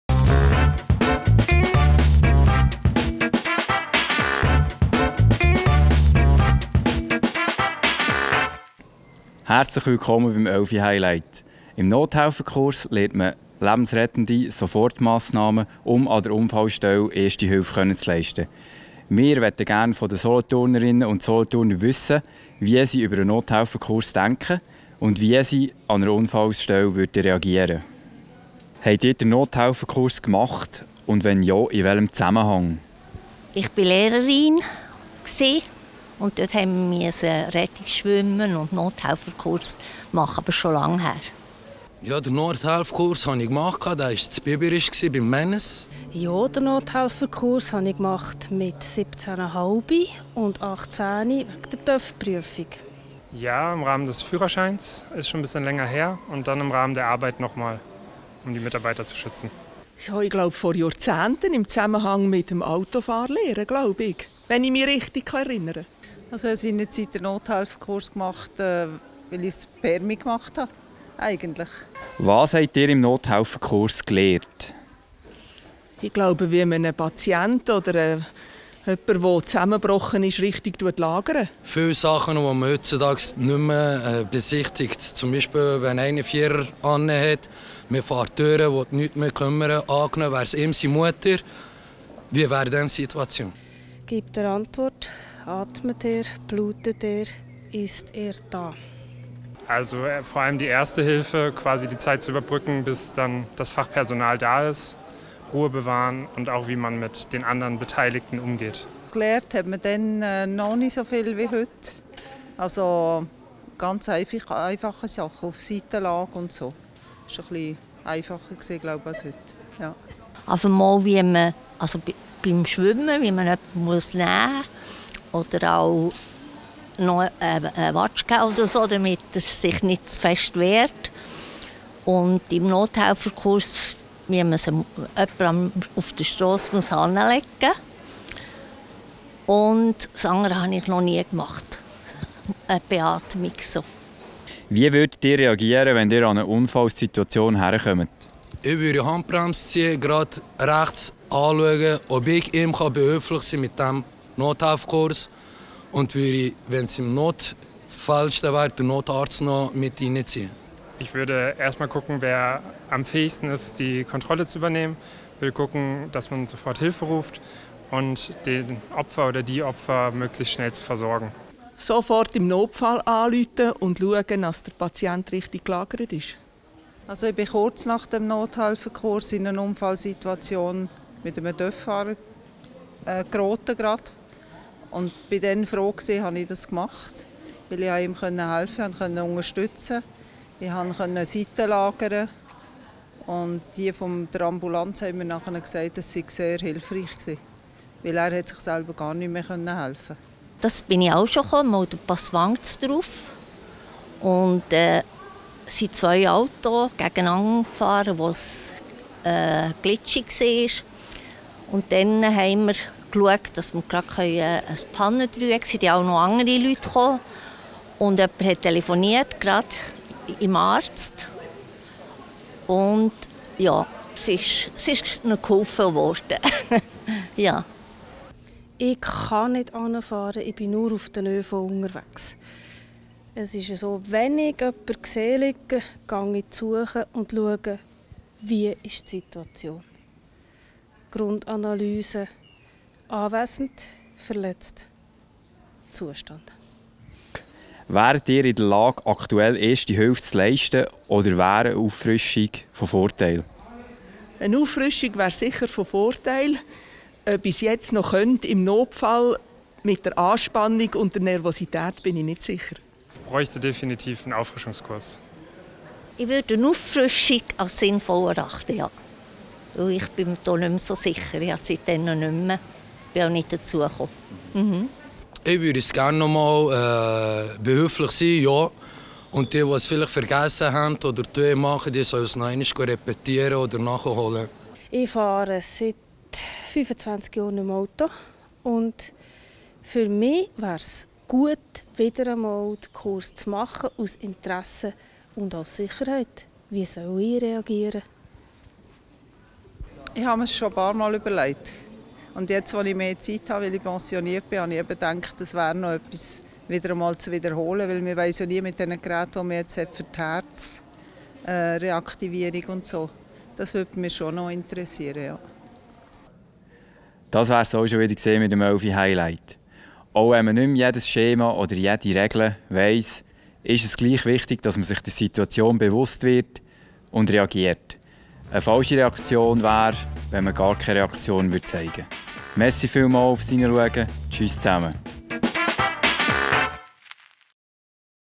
11i-Highlight Nothelferkurs
Erste Hilfe leisten zu können ist ein wichtiges Thema. Im Nothelferkurs erlernt man lebensrettende Sofortmassnahmen um an einer Unfallstelle reagieren zu können. Im Magazin 11i-Highlight wollten wir von den Solothurnerinnen und Solothurnern wissen, was sie über den Nothelferkurs denken und ob sie schon eine Situation erlebt haben, wo das Gelernte angewendet werden musste.